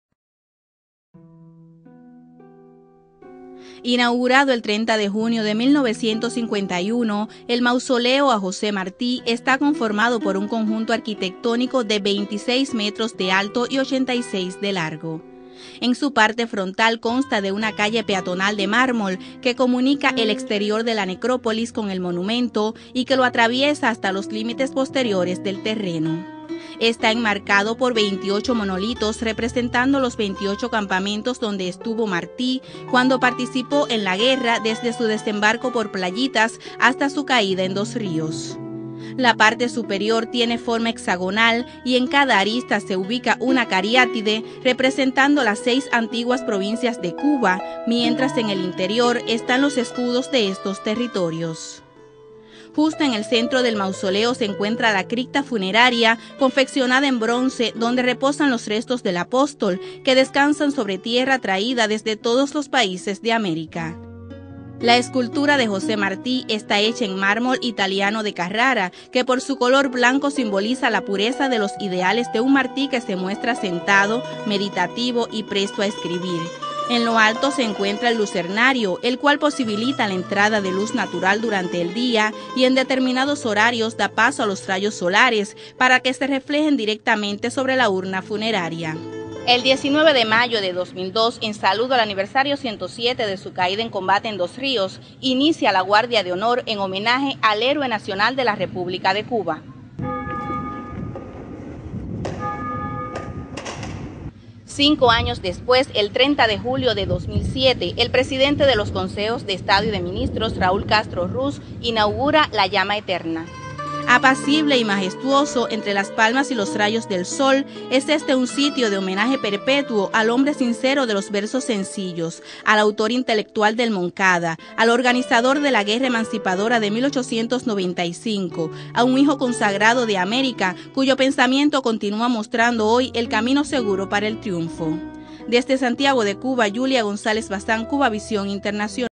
Sitio de homenaje perenne a José Martí
Inaugurado el 30 de junio de 1951 el mausoleo a José Martí está conformado por un conjunto arquitectónico de 26 metros de alto y 86 de largo